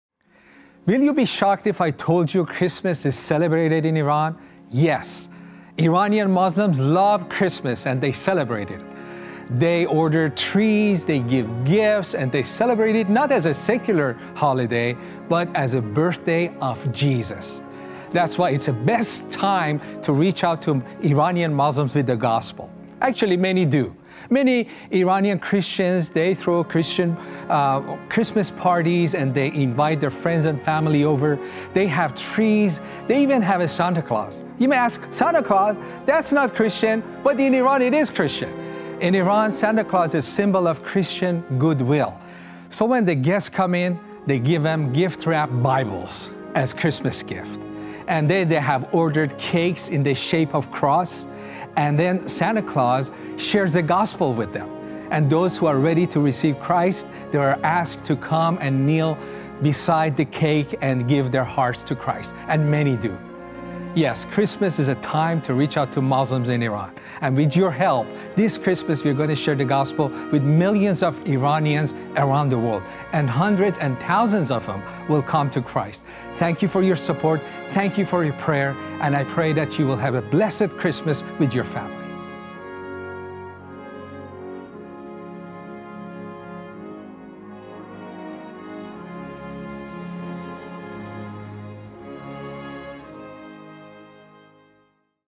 Christmas in Iran - It's not how you imagine it!
Would it surprise you to know Christmas is openly celebrated in Iran? Watch this video to learn about why Christmas is the best time to share the Gospel with Muslims in Iran!